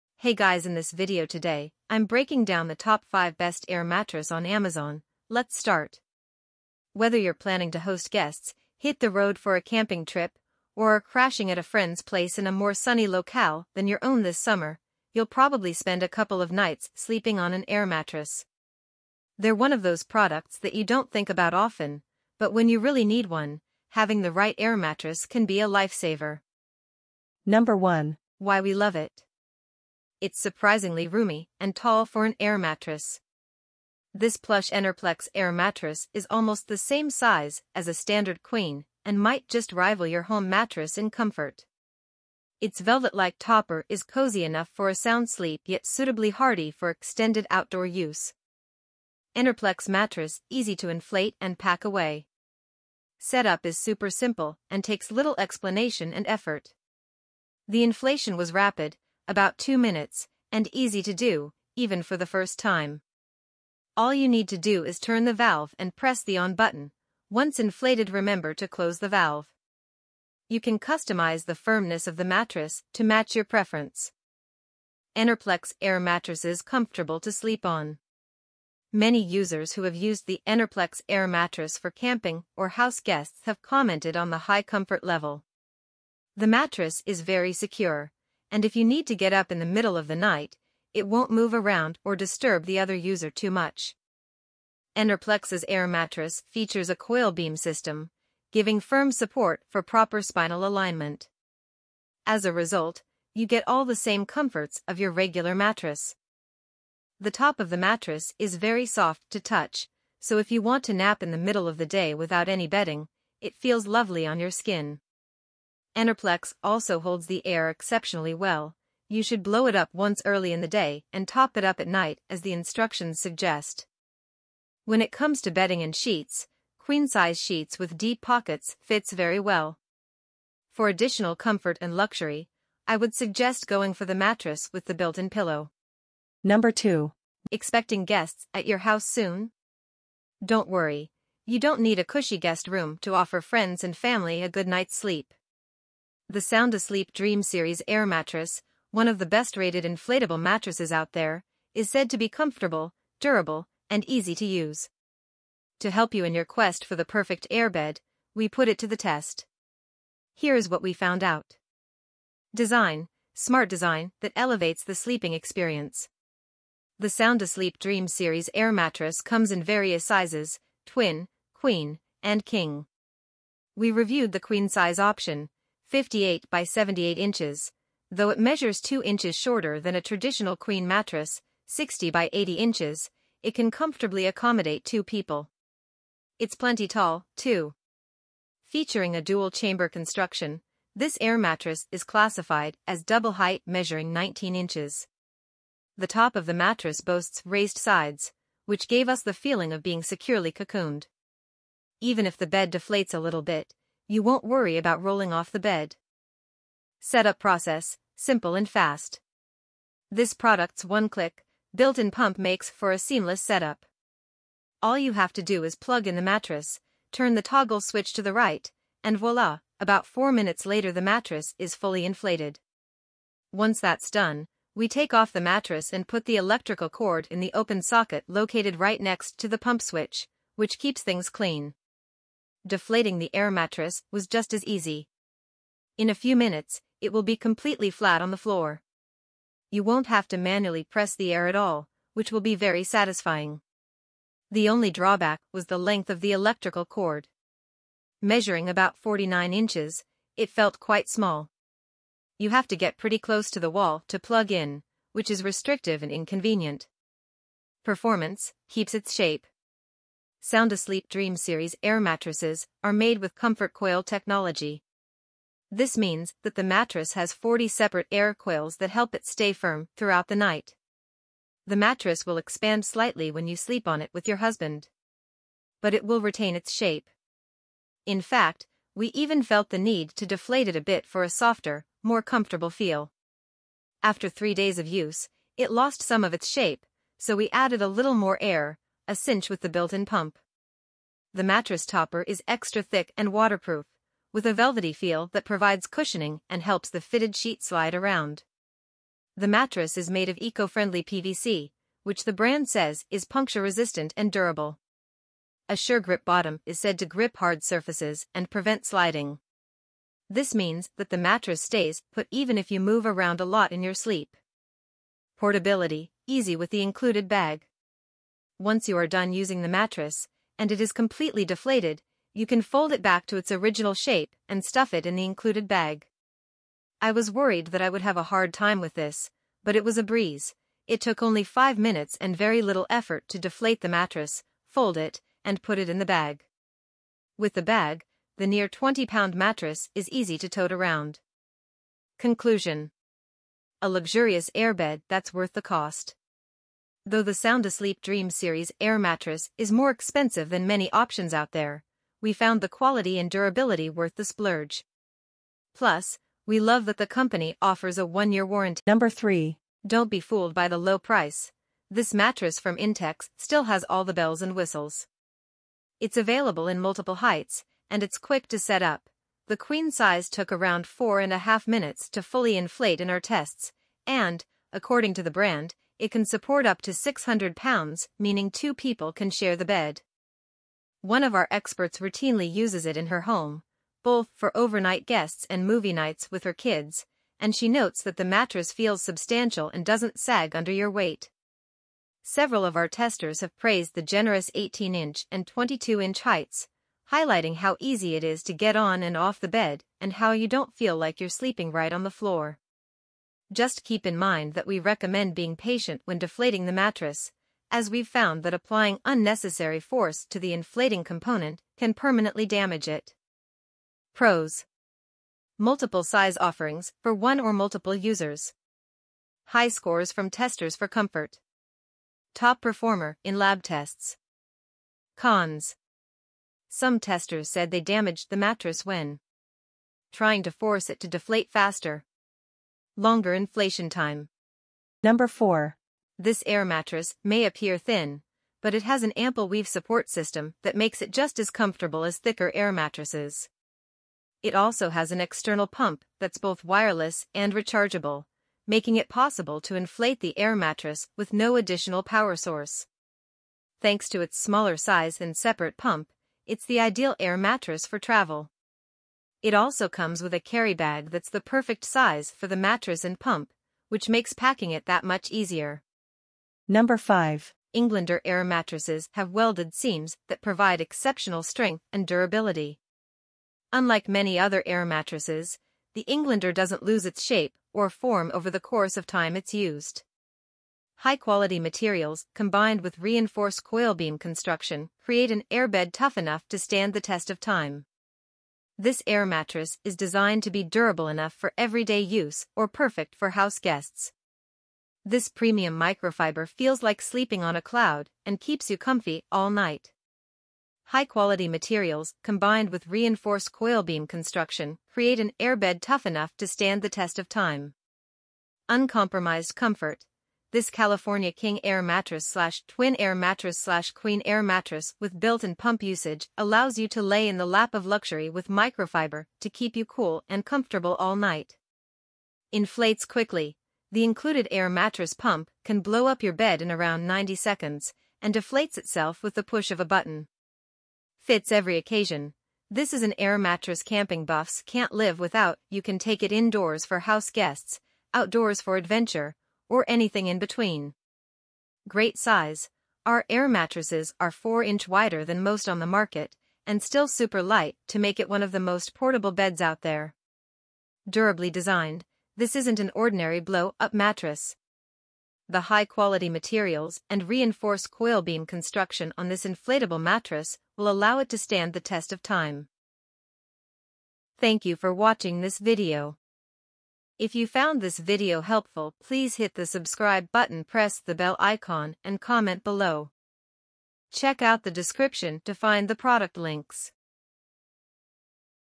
Top 5 Best Air Mattress On Amazon in 2023
In this video we will show Best Air Mattress On Amazon in 2023, EnerPlex Air Mattress, SoundAsleep Dream Series Air Mattress, Intex Dura-Beam Deluxe Comfort Air Mattress, Etekcity Camping Air Mattress, Englander Microfiber Queen Air Mattress

👉 Best 5 Air Mattress Lists:

1)  EnerPlex Air Mattress

2) SoundAsleep Products Dream Series Air Mattress

3) Intex Dura-Beam Deluxe Comfort Air Mattress

 4) Etekcity Camping Air Mattress

5) Englander Microfiber Queen Air Mattress

having a quality Air Mattress is an important factor for maximizing your sleeping quality and some options have advanced technology to deliver a better sleeping experience With so many options to choose from,finding one that fits your budget can be complicated ,so it's important to learn about some of the best options available.

In this video we'll be comparing the top 5 Best Air Mattress on amazon that are designed for different kinds of users. We will take into account performance features and price so you can decide which is best for you. All the products on our list were selected based on their own inherent strengths and features.

We'll be comparing the EnerPlex Air Mattress, SoundAsleep Dream Series Air Mattress, Intex Dura-Beam Deluxe Comfort Air Mattress, Etekcity Camping Air Mattress, Englander Microfiber Queen Air Mattress; which are all great options if you're in the market for a best mattress topper.

We'll break down which Air Mattress is best for you and what you can expect to get in return for your money. We'll help you decide if one of the models on our list seems like a great purchase

0:00 - intro
0:27 - EnerPlex Air Mattress
2:16 - SoundAsleep Dream Series Air Mattress
5:58 - Intex Dura-Beam Deluxe Comfort Air Mattress
7:15 - Etekcity Camping Air Mattress
7:46 - Englander Microfiber Queen Air Mattress